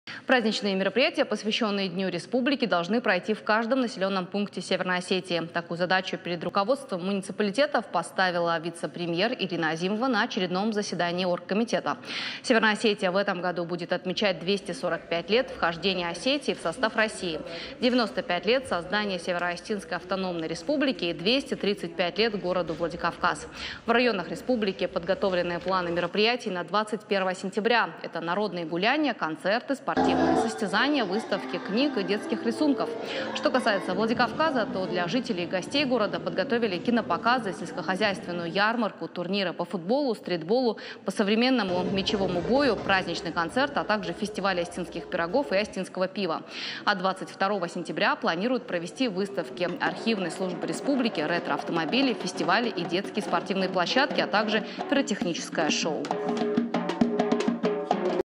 День республики отпразднуют во всех районах Северной Осетии
Праздничные мероприятия, посвященные Дню республики, должны пройти в каждом населенном пункте Северной Осетии. Такую задачу перед руководством муниципалитетов поставила вице-премьер Ирина Азимова на очередном заседании оргкомитета. Северная Осетия в этом году будет отмечать 245 лет вхождения Осетии в состав России, 95 лет создания Северо-Осетинской автономной республики и 235 лет города Владикавказа.

В районах республики подготовлены планы мероприятий на 21 сентября. Это народные гулянья, концерты, спортивные состязания, выставки книг и детских рисунков.

Что касается Владикавказа, то для жителей и гостей города подготовили кинопоказы, сельскохозяйственную ярмарку, турниры по футболу, стритболу и современному мечевому бою, праздничный концерт, а также фестиваль осетинских пирогов и осетинского пива. А 22 сентября планируют провести выставки Архивной службы республики, ретро-автомобилей, фестивали и детские спортивные площадки, а также пиротехническое шоу.
Мы рядом в:
ok.ru/irystontv